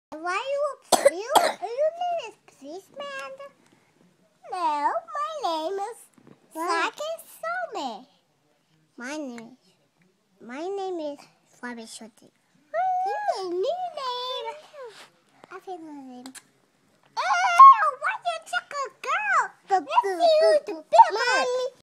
Role play with the girls3
Fevin and Aleena playing with their toys :)